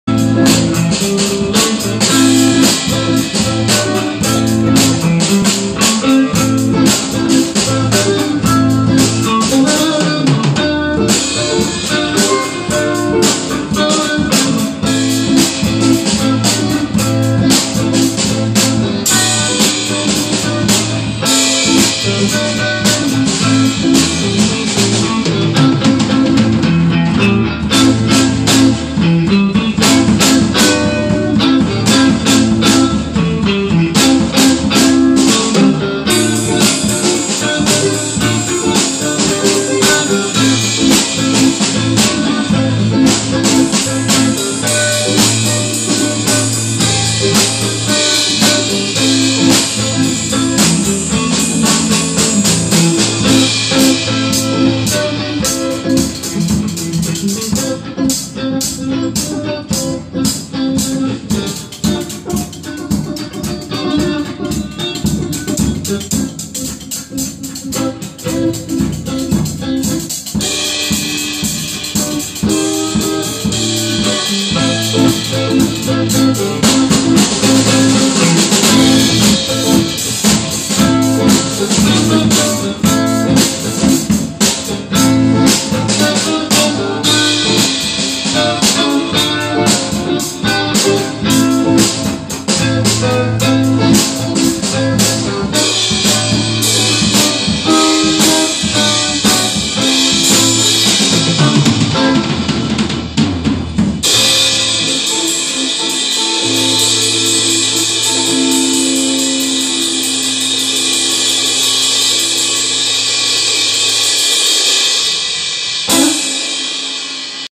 Funk Blues, Before Breakfast
My 10 year old son on drums and I on guitar. On this day in July, before breakfast, this was our morning exercise. Lol.